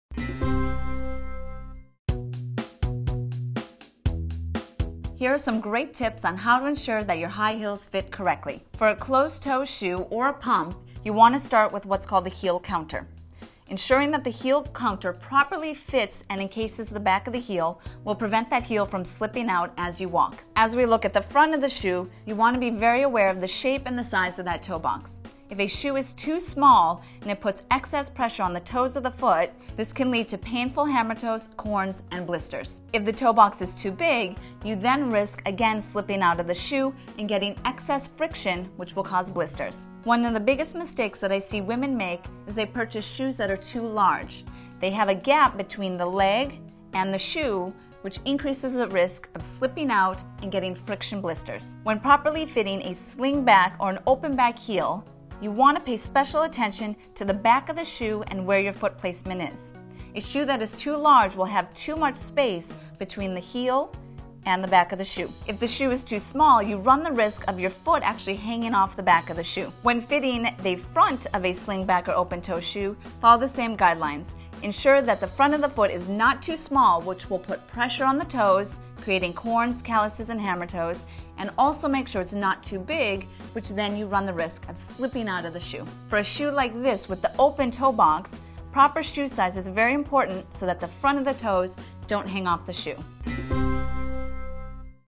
How to Make Sure Heels Fit Correctly | High Heel Walking
Here are some great tips on how to ensure that your high heels fit correctly. For a closed toe shoe or pump, you want to start with what's called the heel counter. Ensuring that the heel counter properly fits, and in cases the back of the heel, will prevent that heel from slipping out as you walk. As we look at the front of the shoe, you want to be very aware of the shape and the size of that toe box.

If a shoe is too small, and it puts excess pressure on the toes of the foot, this can lead to painful hammertoes, corns, and blisters. If the toe box is too big, you then risk again slipping out of the shoe and getting excess friction, which will cause blisters.

One of the biggest mistakes that I see women make, is they purchase shoes that are too large. They have a gap between the leg and the shoe, which increases the risk of slipping out and getting friction blisters. When properly fitting a sling-back or open-back heel, you want to pay special attention to the back of the shoe and where your foot placement is.

A shoe that is too large will have too much space between the heel and the back of the shoe. If the shoe is too small, you run the risk of your foot actually hanging off the back of the shoe. When fitting the front of a sling-back or open-toe shoe, follow the same guidelines. Ensure that the front of the foot is not too small, which will put pressure on the toes, creating corns, calluses and hammertoes. And also make sure it is not too big, which then you run the risk of slipping out of the shoe.

For a shoe like this, with the open toe box, proper shoe size is very important, so that the front of the toes don't hang off the shoe.